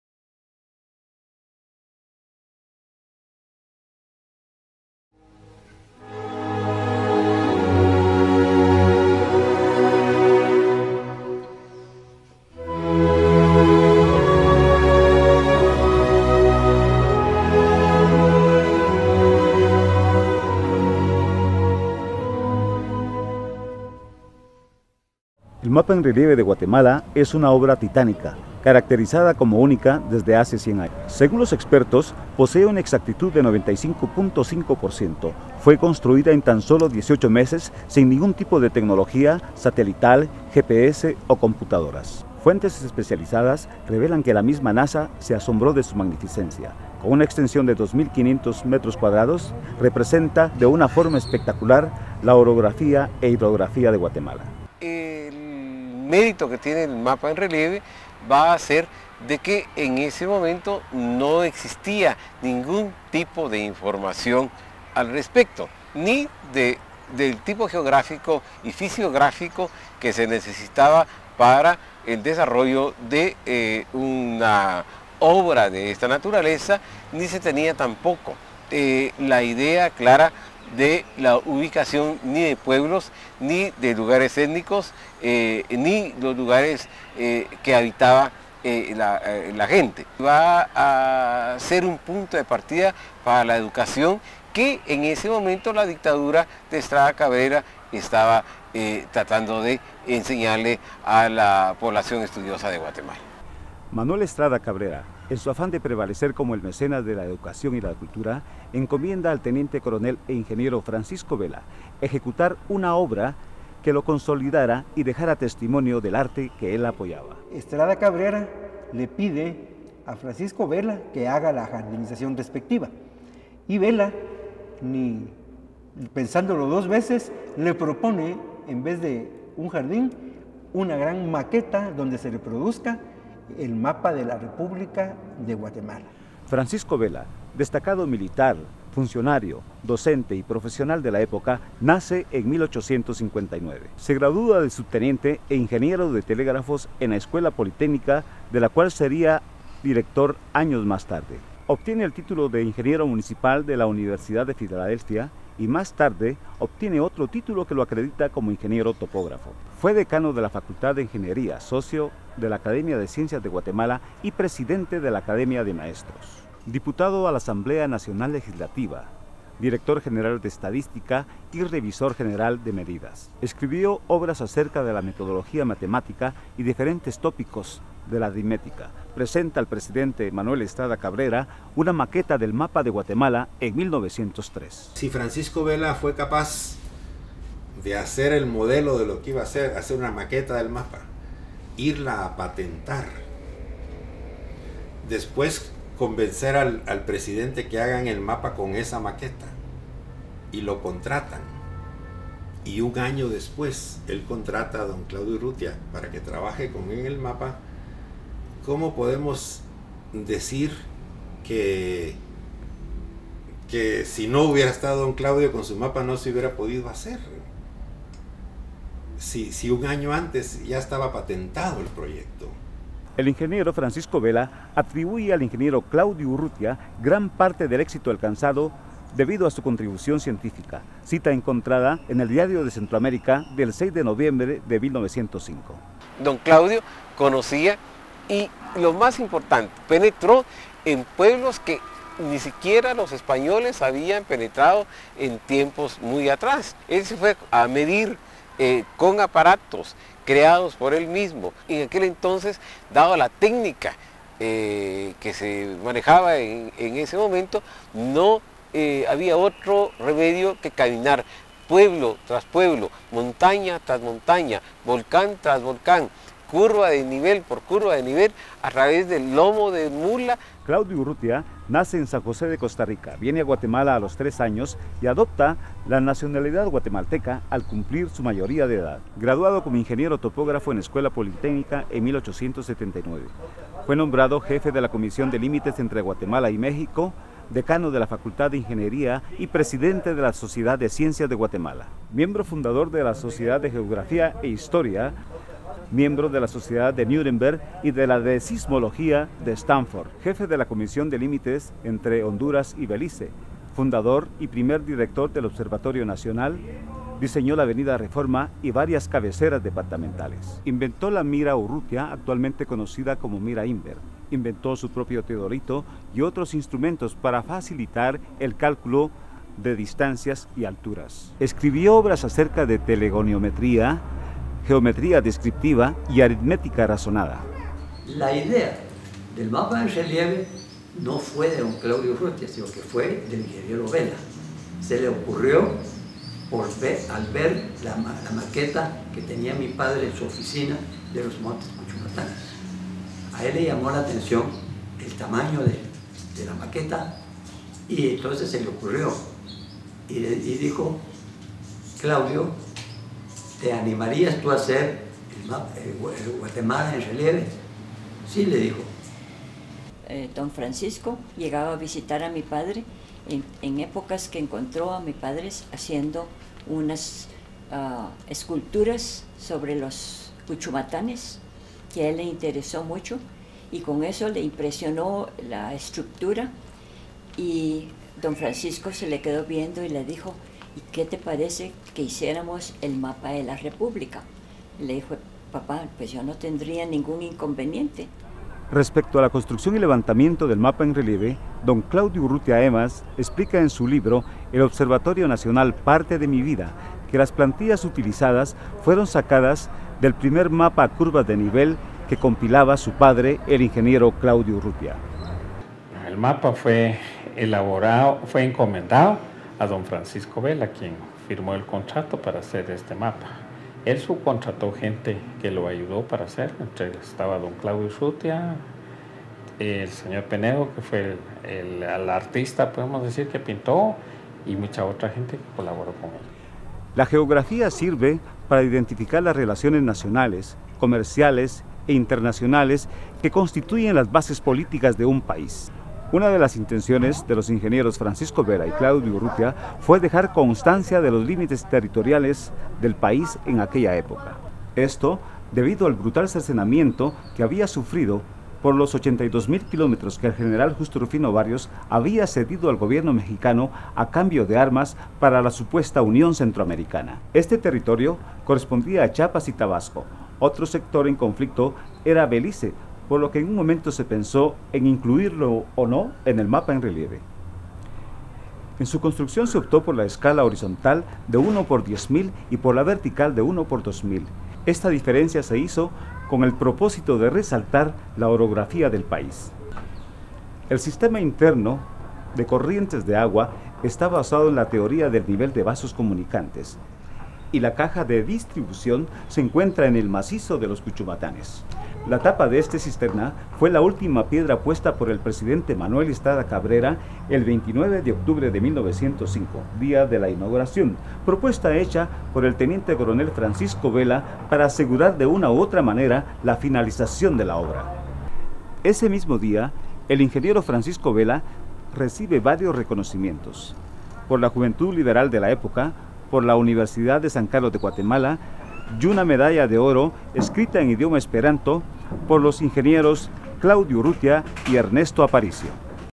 Reportaje TV Centenario Mapa en Relieve Parte 1
Un breve relato historico, contado por Historiadores e Ingenieros;  sobre la construccion y autoria del mapa en Relive por los ingenieros Claudio Urrutia y Francisco Vela finales del siglo 19 y principios del siglo 20 en el gobierno del presidente Estrada Cabrera.